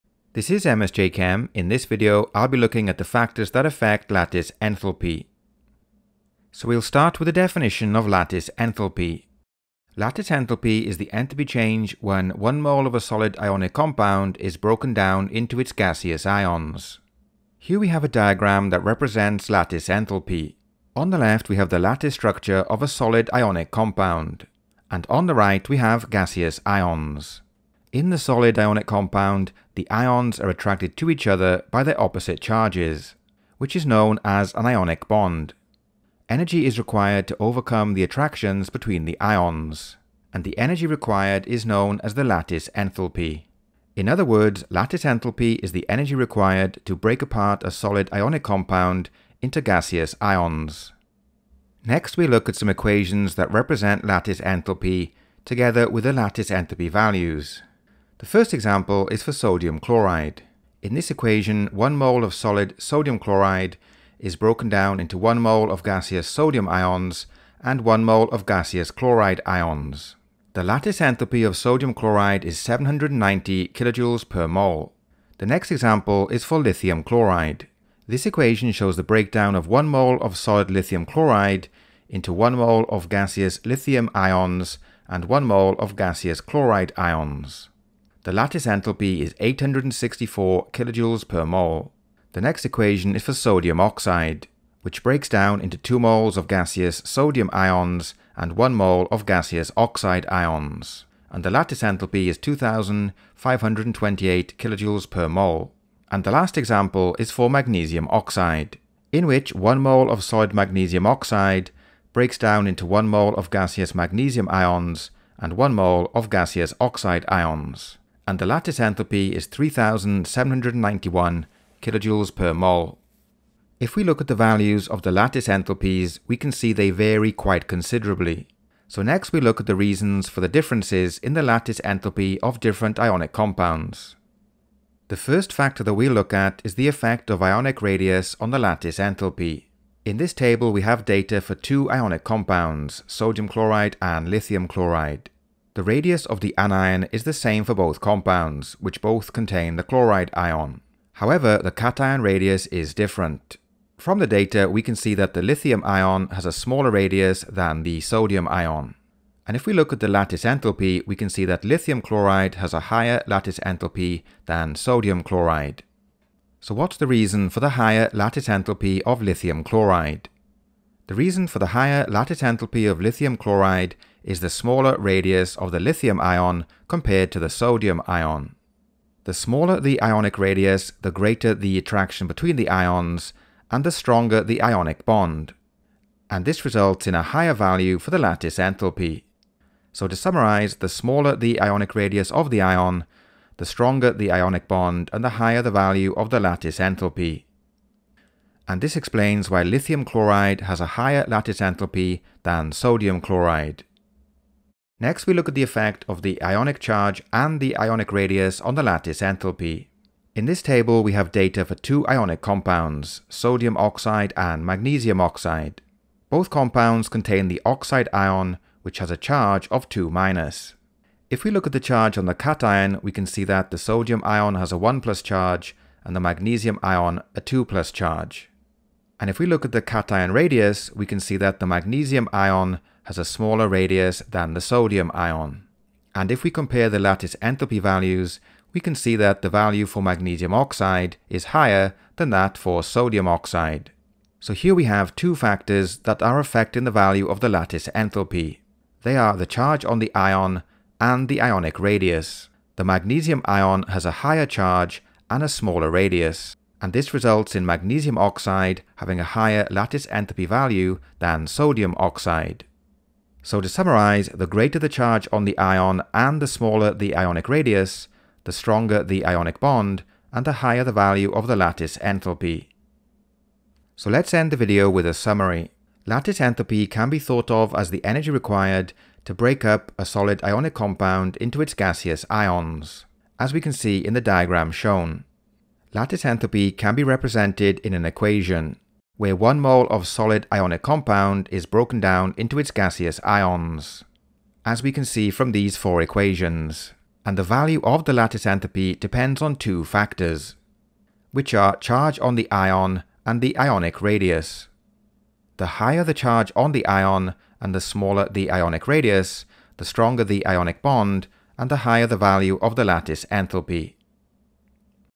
S2.1.3 Factors that affect lattice enthalpy
This video covers the factors that affect the lattice enthalpy of ionic compounds.